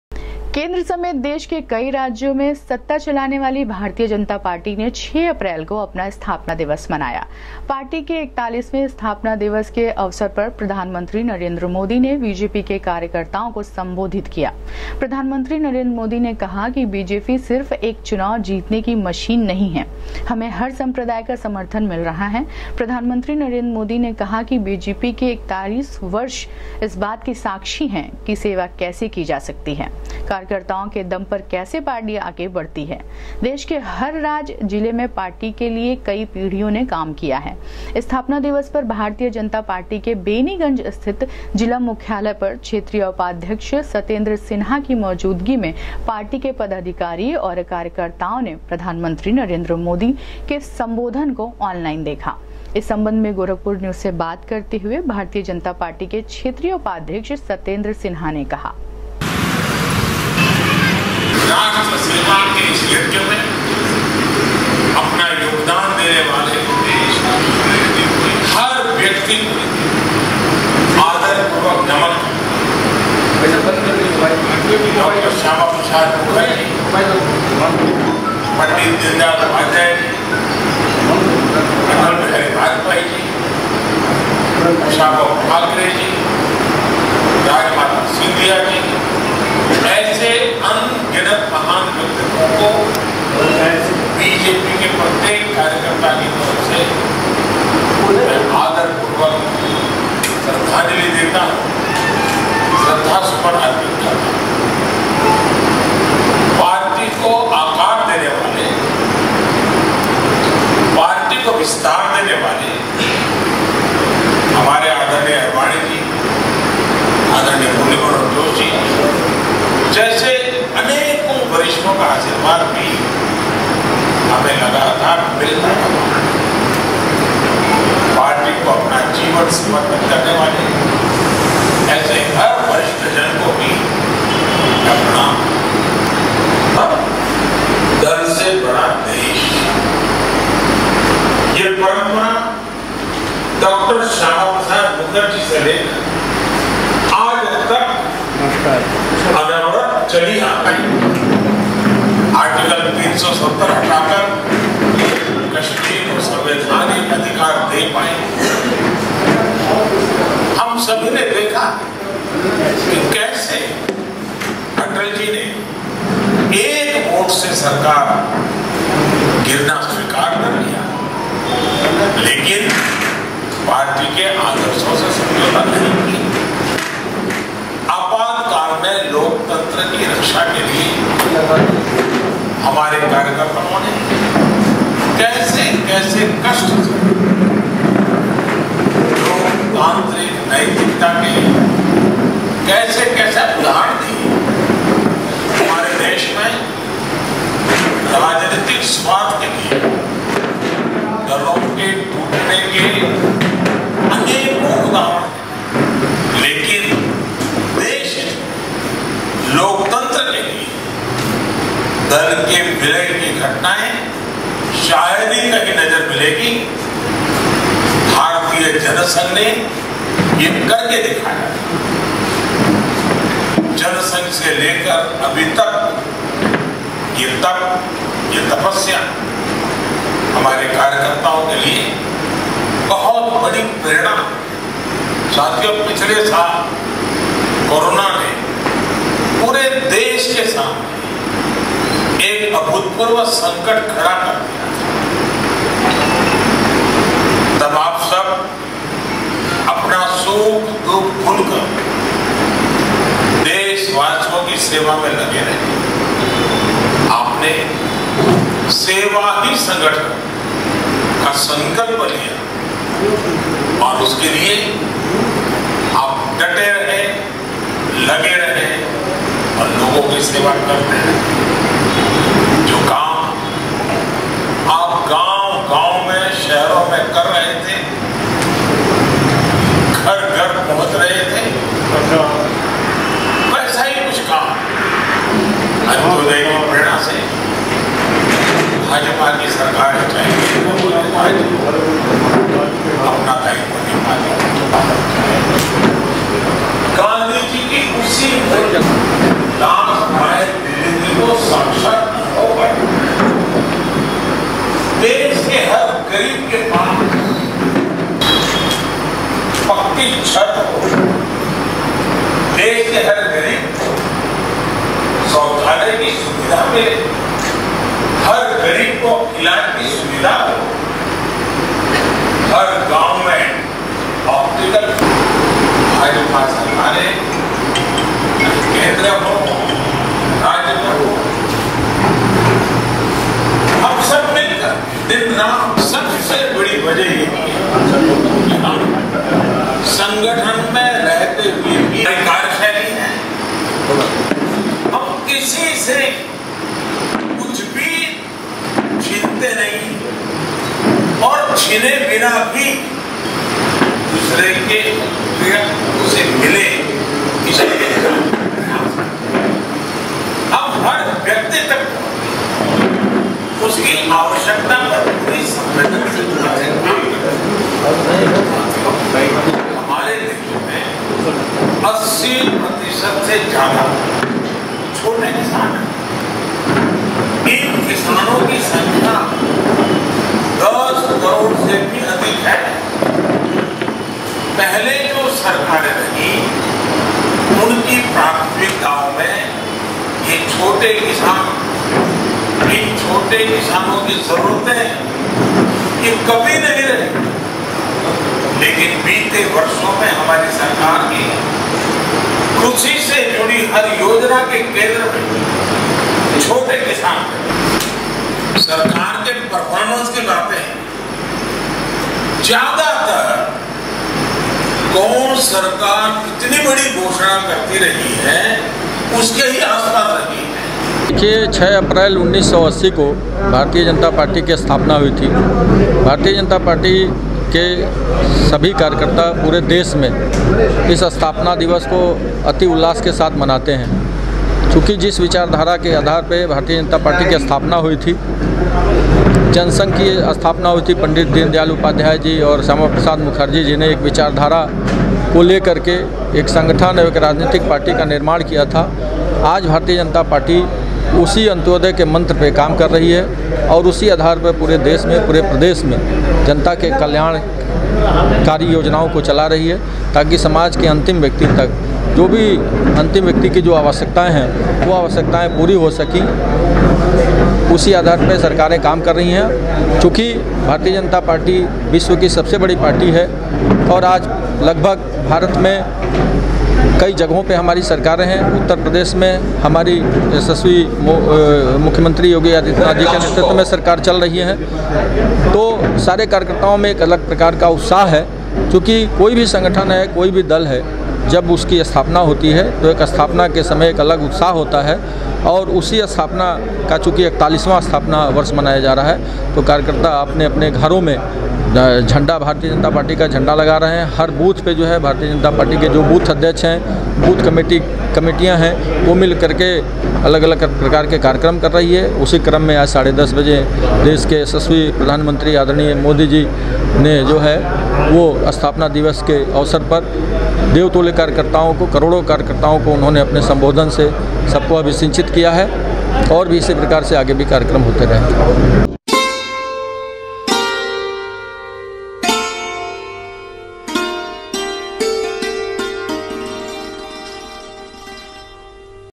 बीजेपी के 41 वें स्थापना दिवस पर देश भर के कार्यकर्ताओं को पीएम मोदी ने किया संबोधित, जिला मुख्यालय
बीजेपी के 41 वें स्थापना दिवस पर देश भर के कार्यकर्ताओं को पीएम मोदी ने किया संबोधित, जिला मुख्यालय पर कार्यकर्ताओं ने सुना संबोधन